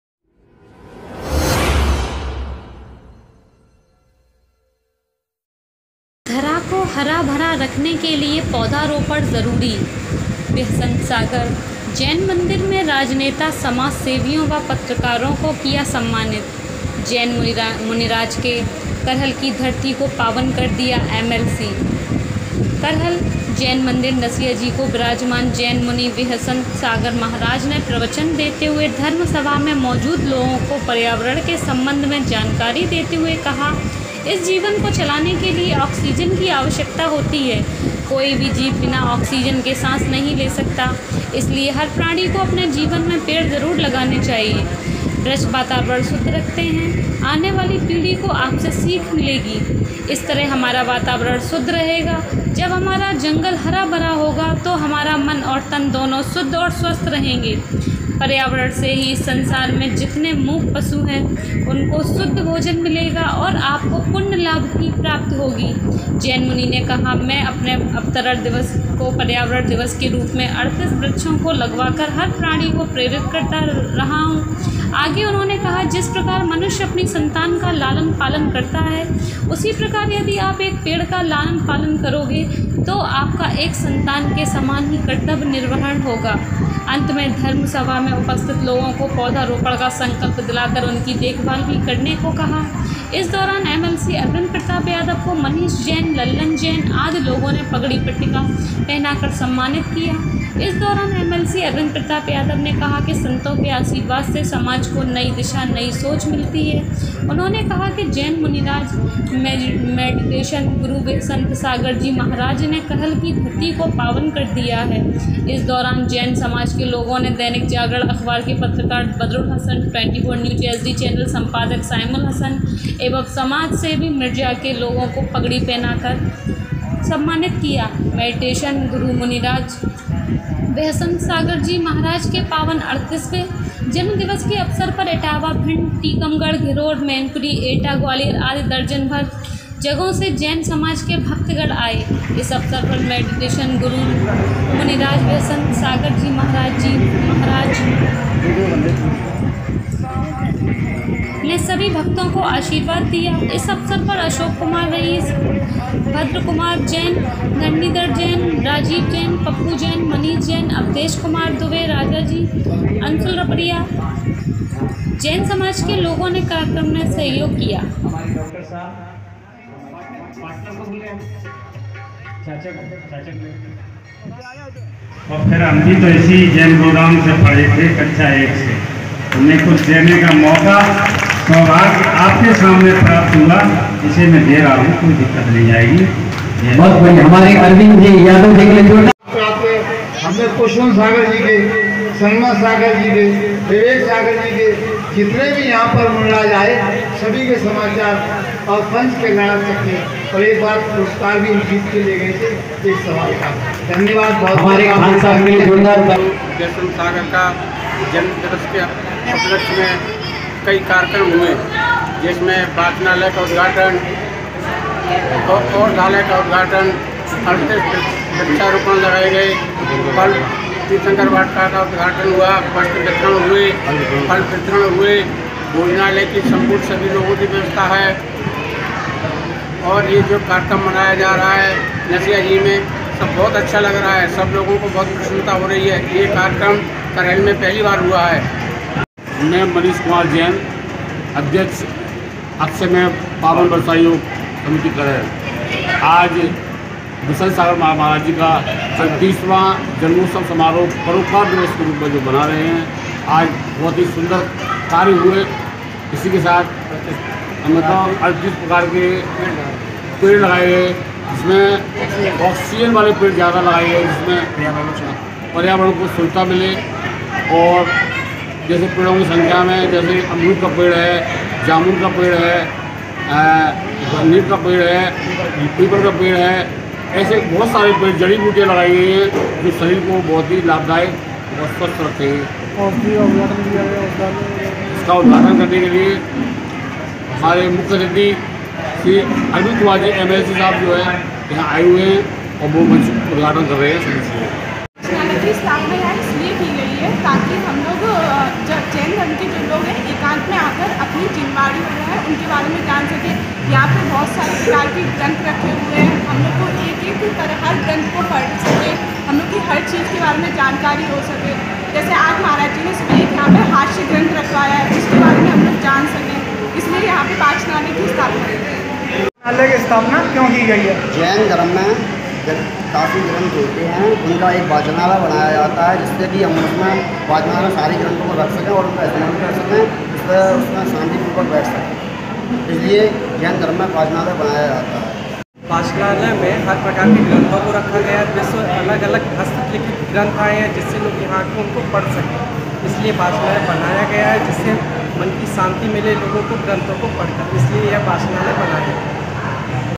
मेडिटेशन गुरु विहंसत सागर जी महाराज के 38 वें जन्मदिवस पर नसिया जी मंदिर में हुए 12 प्रोग्राम
करहल के नसिया जी जैन मंदिर में यूपी मैनपुरी करहल मेडिटेशन_गुरु_विहसंत सागर